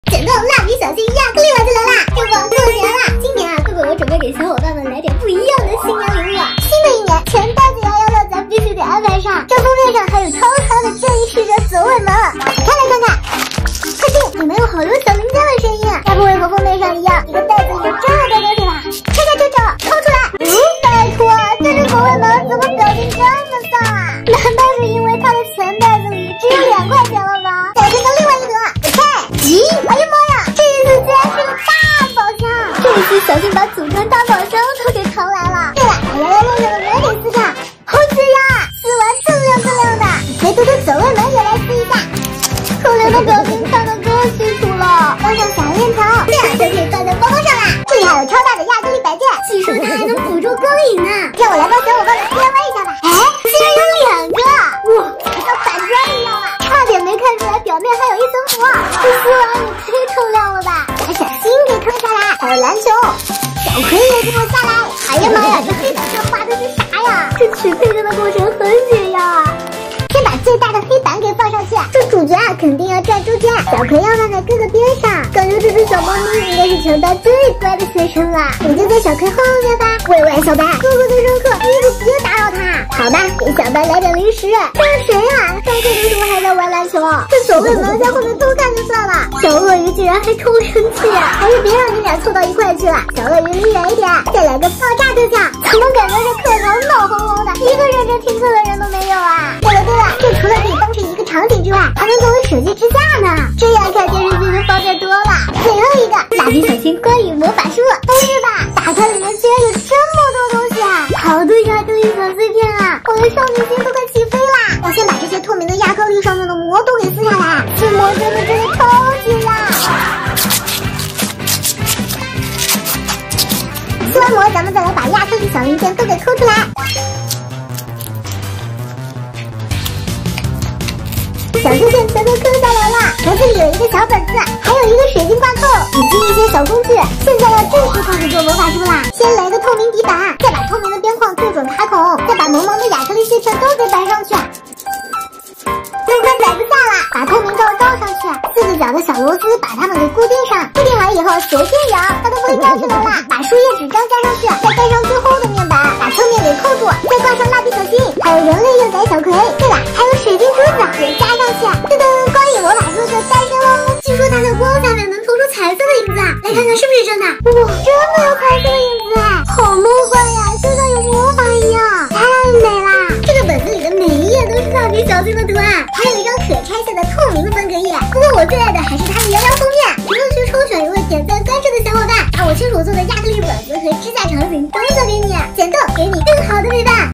简动蜡笔小新亚克力三件套！钱袋摇摇VS教室场景，光影魔法书超美
歡迎來到桂圓子的創意小世界💗
這裡桂桂會分享好玩有趣的手作DIY史萊姆給寶寶們